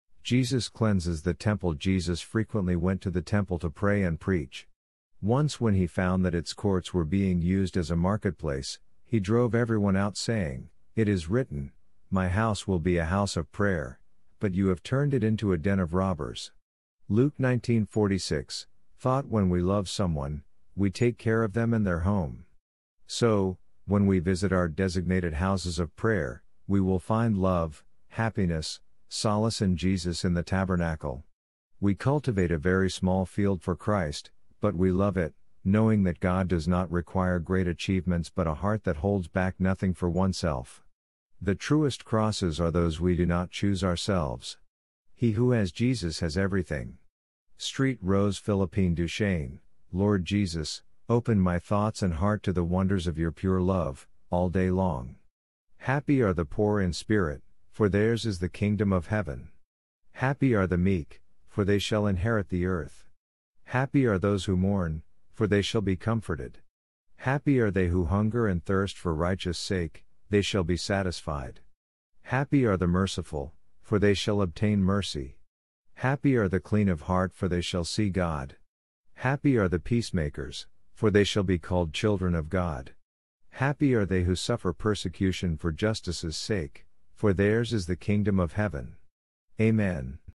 Jesus Cleanses The Temple
Jesus frequently went to the Temple to pray and preach.
 Once when he found that its courts were being used as a marketplace, he drove everyone out saying, “It is written, ‘My house will be a house of prayer,’ but you have turned it into a den of robbers”. (Luke 19:46)
 Thought
 When we love someone, we take care of them and their home. So, when we visit our designated Houses of Prayer, we will find love, happiness, solace and Jesus in the Tabernacle.
 “We cultivate a very small field for Christ, but we love it, knowing that God does not require great achievements but a heart that holds back nothing for oneself…. The truest crosses are those we do not choose ourselves…. He who has Jesus has everything”. (St. Rose Philippine Duchesne)
 Lord Jesus, open my thoughts and heart to the wonders of your pure love, all day long.
 Happy are the poor in spirit, for theirs is the kingdom of heaven.Happy are the meek, for they shall inherit the earth.Happy are those who mourn, for they shall be comforted.Happy are they who hunger and thirst for righteous sake, they shall be satisfied.Happy are the merciful, for they shall obtain mercy.Happy are the clean of heart for they shall see God.Happy are the peacemakers, for they shall be called children of God.Happy are they who suffer persecution for justice’s sake, for theirs is the kingdom of heaven. Amen.